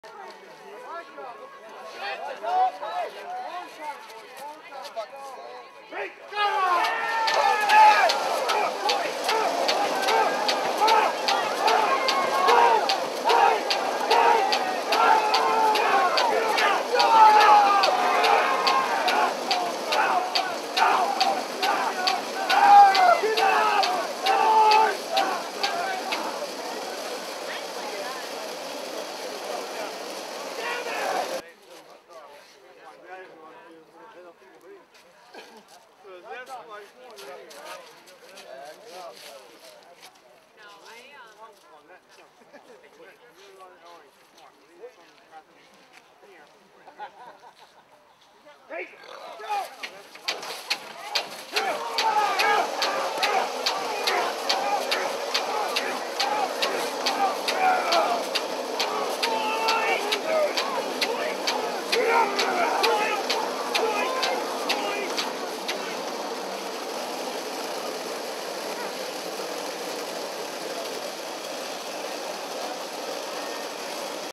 Protection at the Woolwich Muster
The Protection no. 1 at the New England States Veteran Fireman's League Muster in Woolwich, Maine on September 12, 2009. This competition is to see who can  throw a water stream the longest with an an antique fire engine, also known as a hand tub.